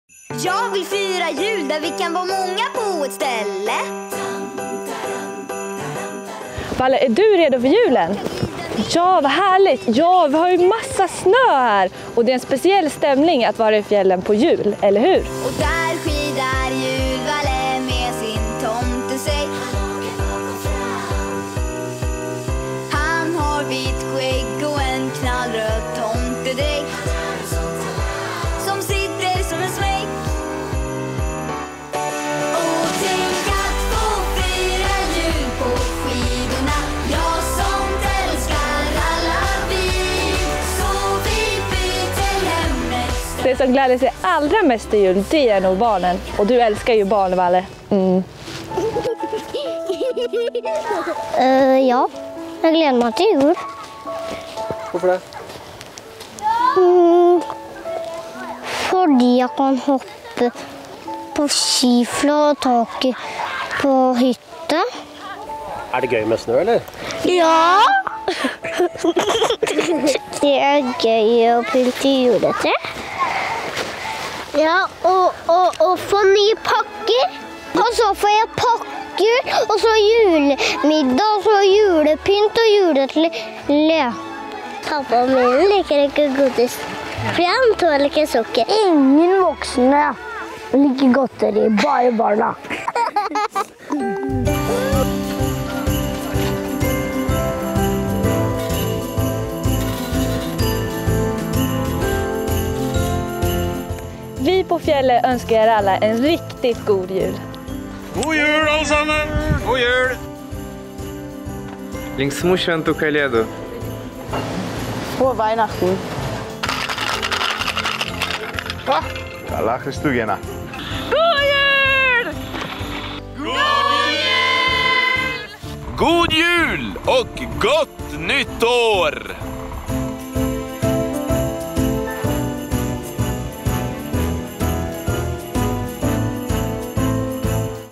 Valle med vänner önskar God Jul
Valle älskar julen, hans vänner med! De har gett sig ut i skidområdet för att se om de kan hitta någon julstämning. Valle träffar barn som längtar efter julklappar, tomten och som älskar snö. Skidskola, maskinförare och hotellpersonal passar också på att önska en riktigt God Jul!